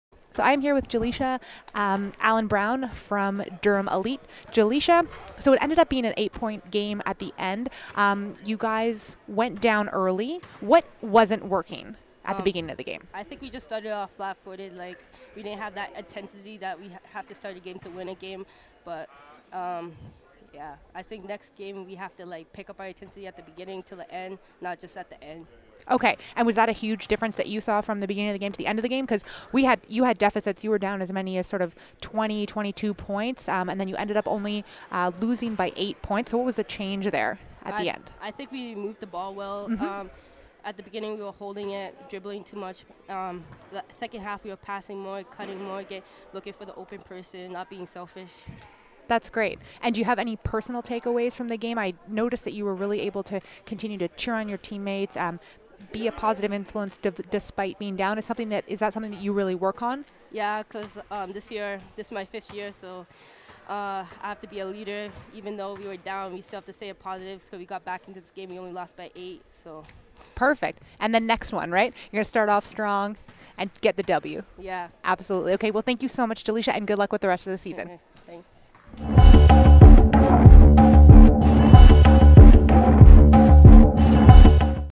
2019 5’10” G Jolesha Allen-Brown with 5 pts in Durham Elite's loss to King's Christian
King Christian's Jolesha Allen-Brown sat down with OSBA TV's Bess Lennox to discuss her team’s loss to Durham Elite